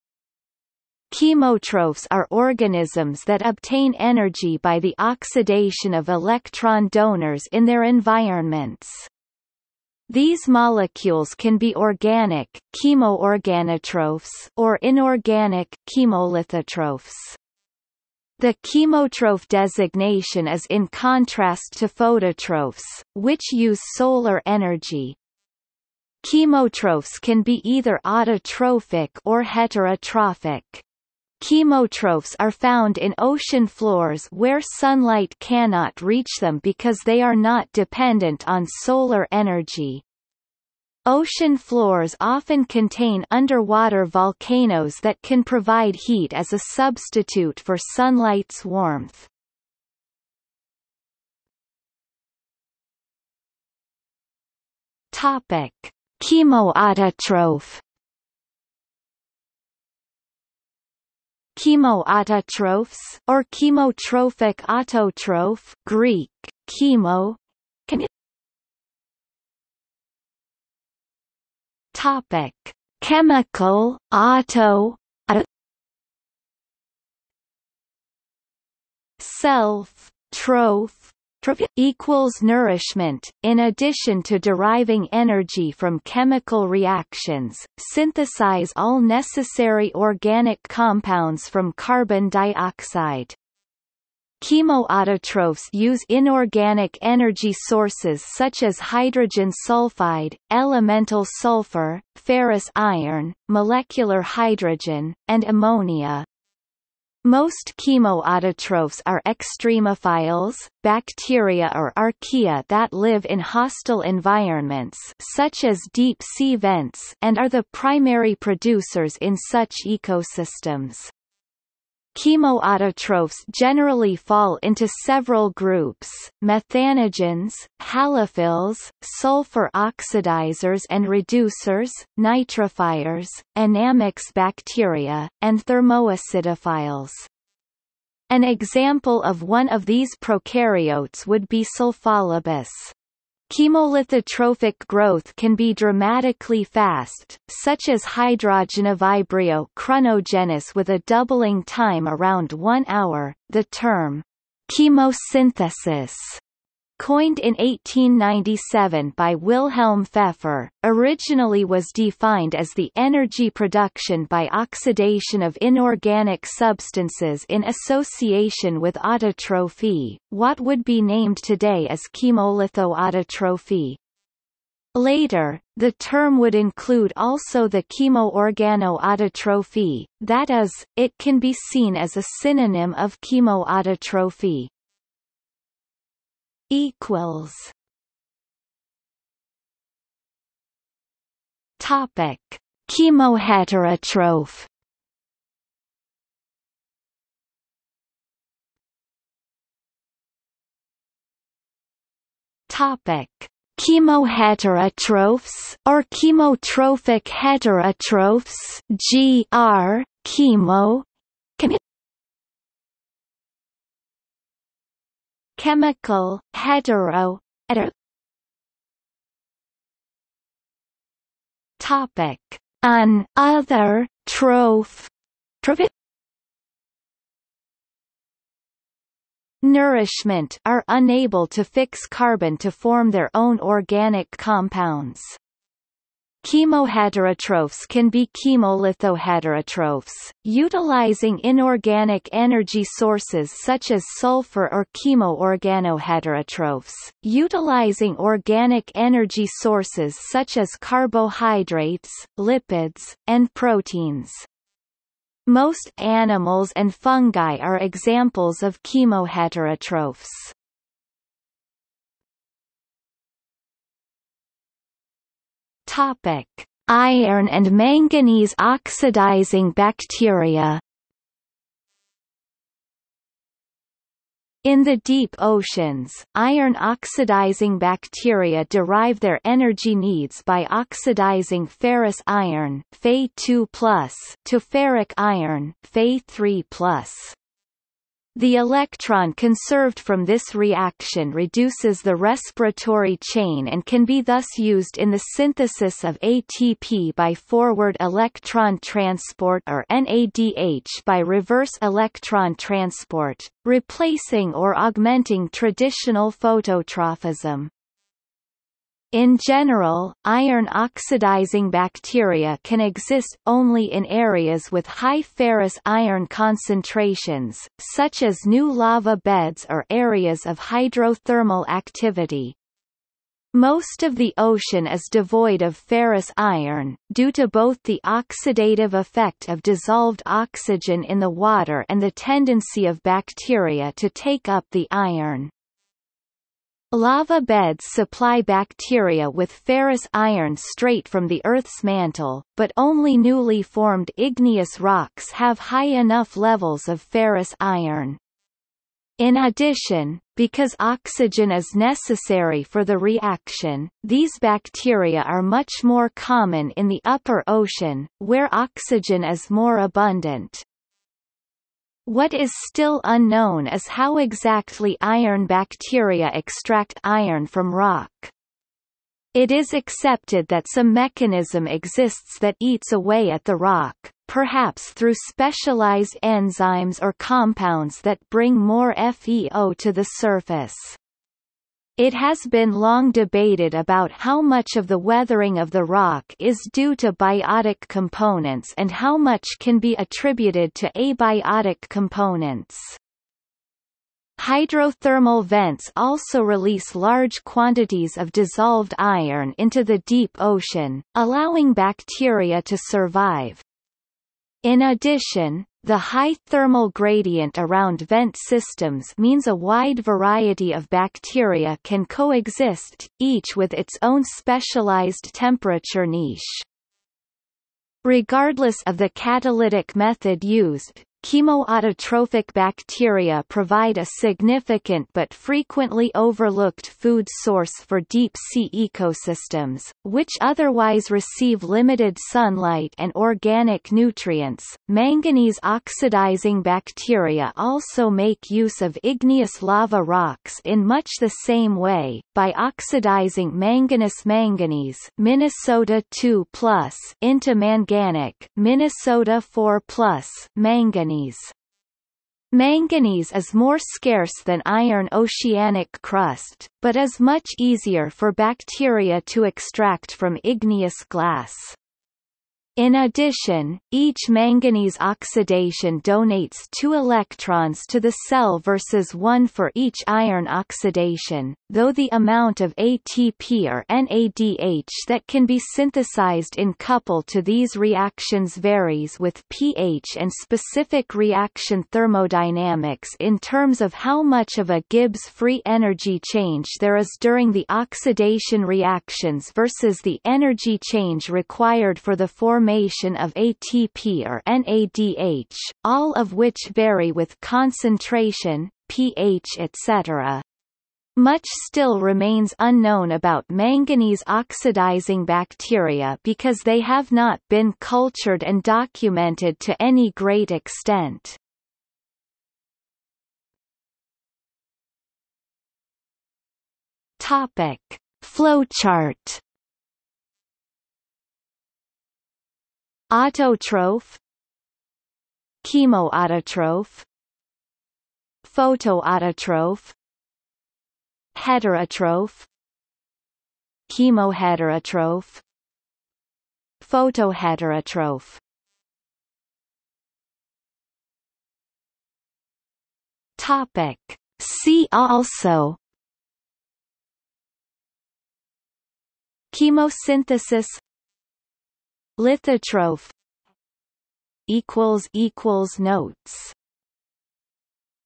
Chemotrophic | Wikipedia audio article
00:00:51 1 Chemoautotroph
00:01:08 2 Chemoheterotroph
00:01:21 3 Iron- and manganese-oxidizing bacteria
00:03:07 4 Flowchart
00:03:18 5 See also
00:03:37 6 Notes
00:04:22 7 References
00:08:33 Flowchart
00:08:59 See also

- Socrates

SUMMARY
Chemotrophs are organisms that obtain energy by the oxidation of electron donors in their environments. These molecules can be organic (chemoorganotrophs) or inorganic (chemolithotrophs).  The chemotroph designation is in contrast to phototrophs, which use solar energy. Chemotrophs can be either autotrophic or heterotrophic. Chemotrophs are found in ocean floors where sunlight cannot reach them because they are not dependent on solar energy. Ocean floors often contain underwater volcanos that can provide heat as a substitute for sunlight's warmth.